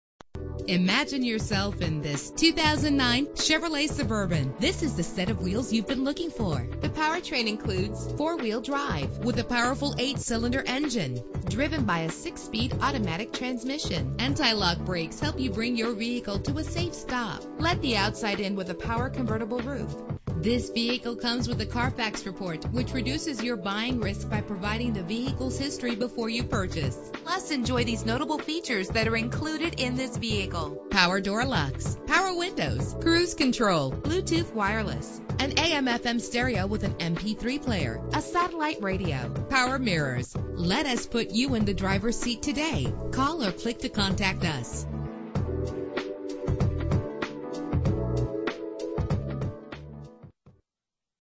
2009 Chevrolet Suburban - Brunswick ME
This vehicle represents similar vehicles available at Goodwin Chevrolet Mazda.
for availability of specific vehicles.

Phone:
Year: 2009
Make: Chevrolet
Model: Suburban
Trim: 1500 LT
Engine: 5.3 liter 8 cylinder 16 valve MPFI OHV Flexible Fuel
Transmission: 6-Speed Automatic
Color: Black
Mileage: 62533
Address:
195 Pleasant St